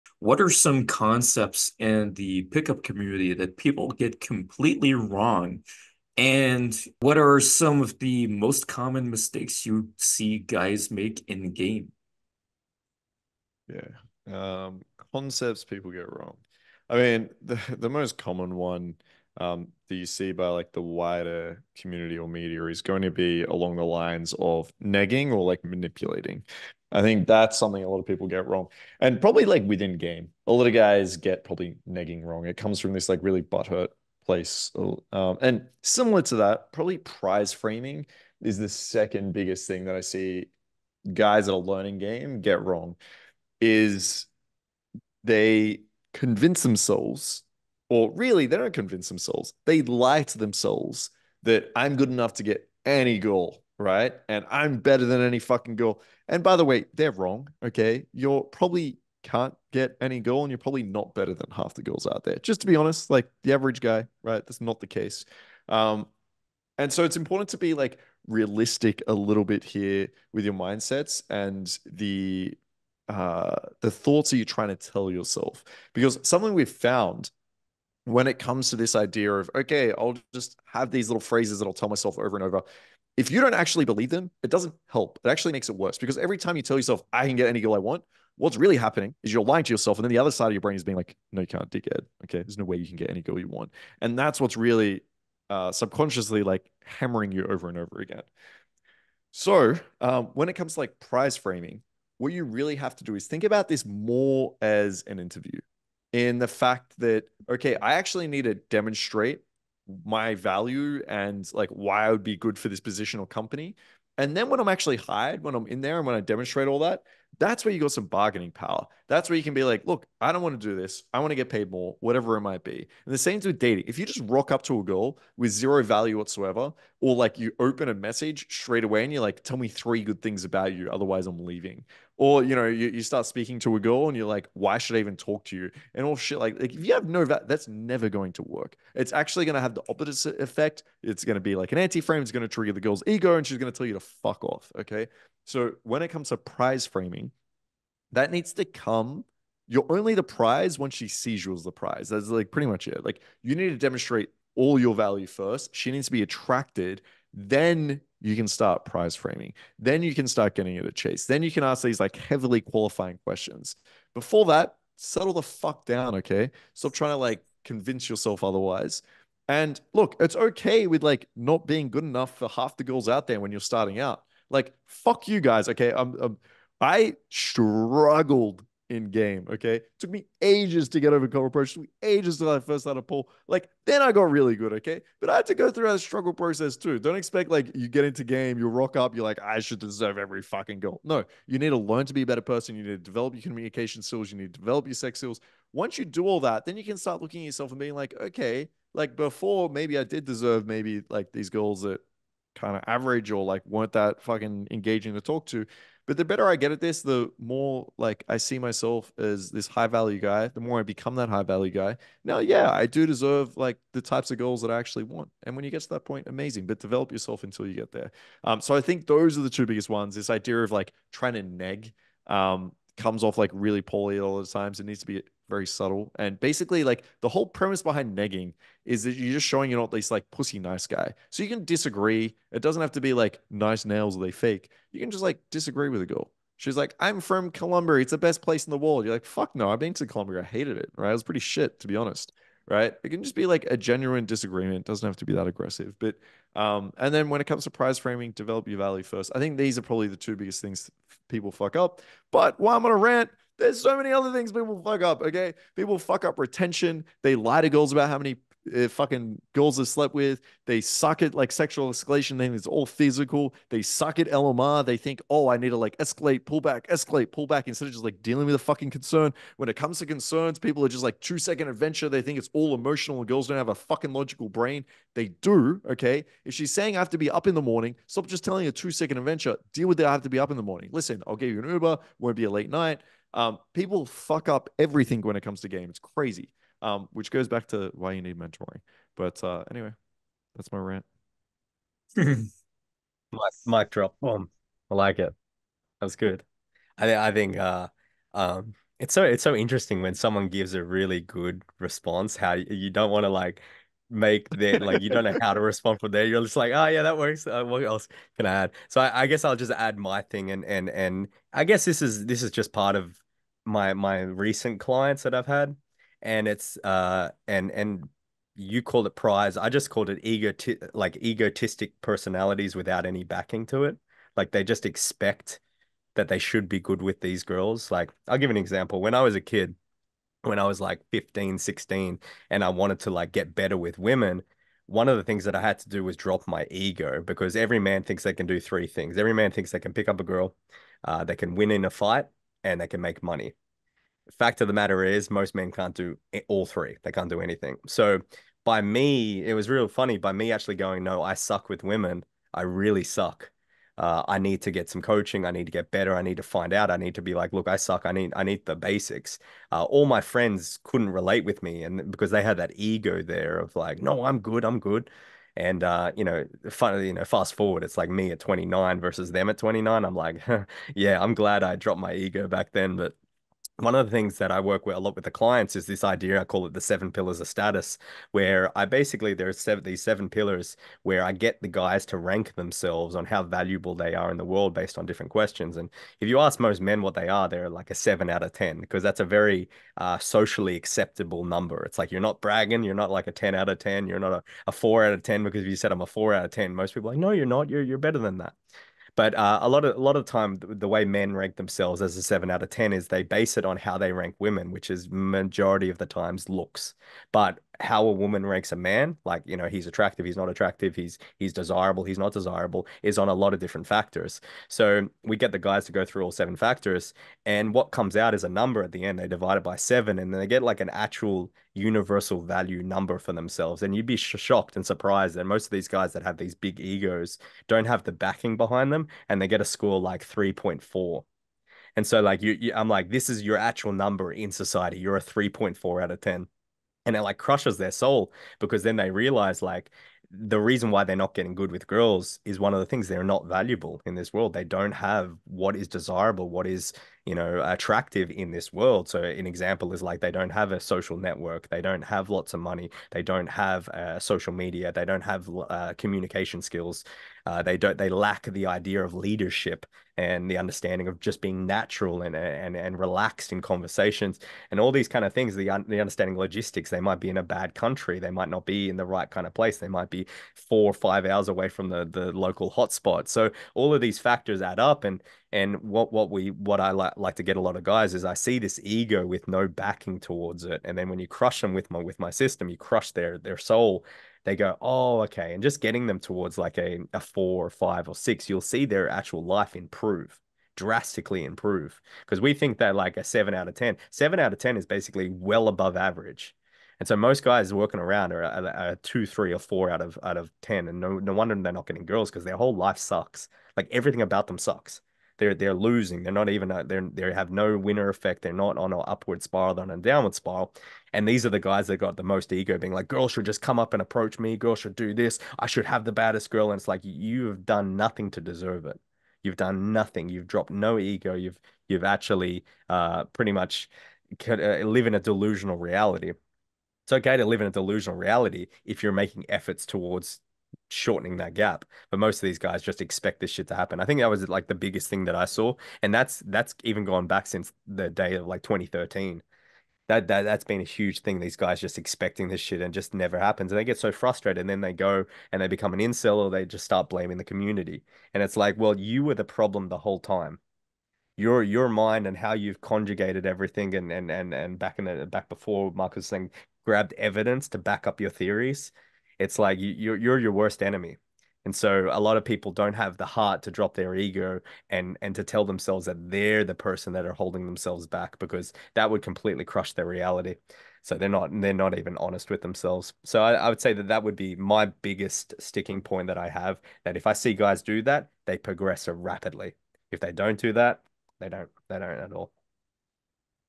Markus Wolf Epic Rant: What Pickup Artists Are Doing Wrong [Ice White] [@UltimateManProject]
Markus Wolf (Ultimate Man Project) goes on an epic rant about what pickup artists are doing wrong. This clip comes from the Dating Coach Panel:

12 Secrets About Attracting Women Society Won’t Tell You

See how we can best help you reach your dating goals